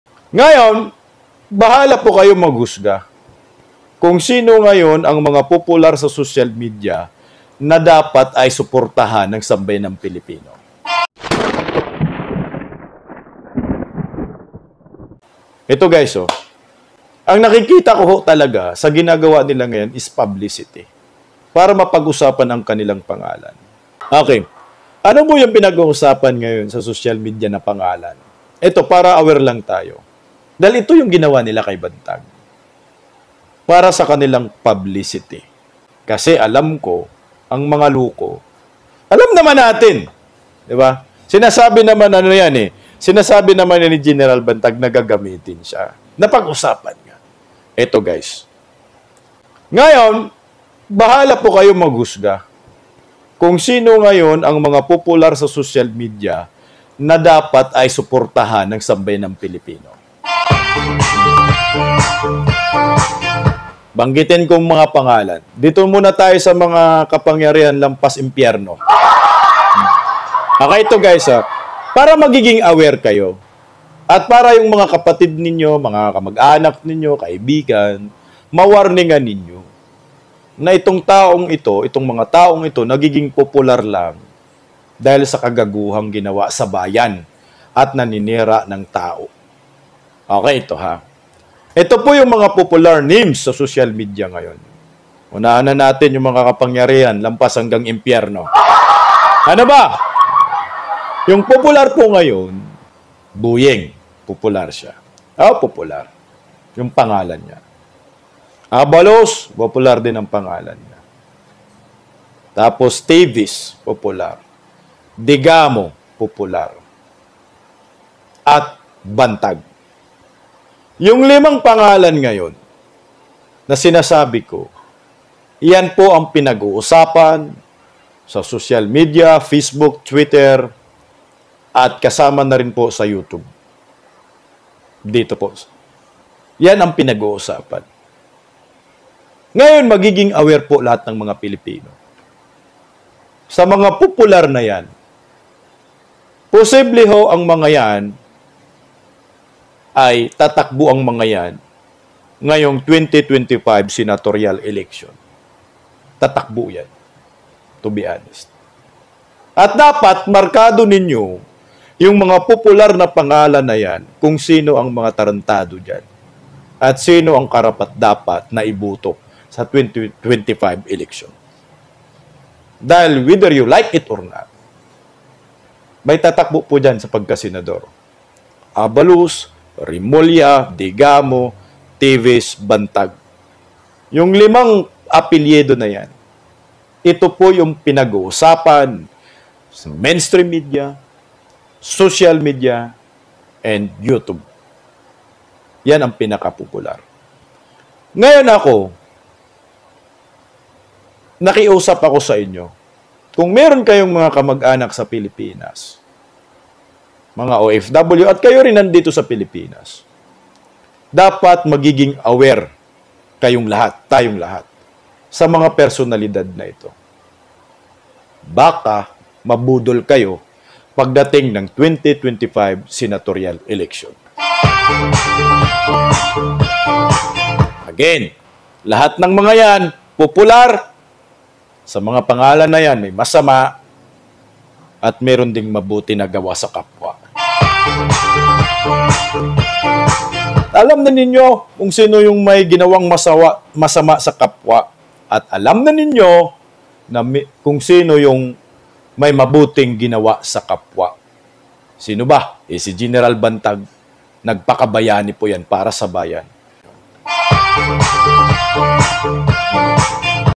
MGA SIKAT SA SOCMED NA POSIBLENG TATAKBONG SENADOR SINO ANG IBOBOTO MO? BANTAG TAYO!
Ang mga pangalang popular sa socmed: Bantag, Remulla, Abalos, Teves at Degamo paniguradong may tatakbong Senador sa mga yan. Huwag magpabudol basta kami si Bantag ang pambato namin sa Senado sa 2025.